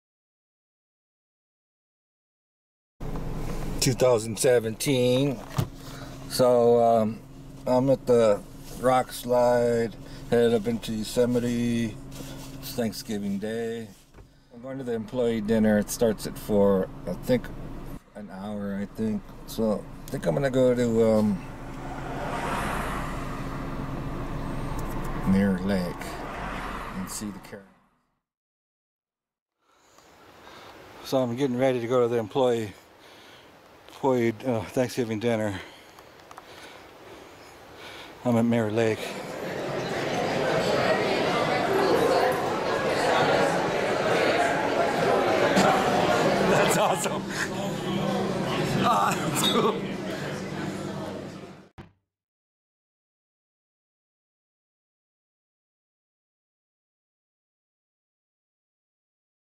Yosemite Thanksgiving 17
I drove up to Yose ite for the employee dinner.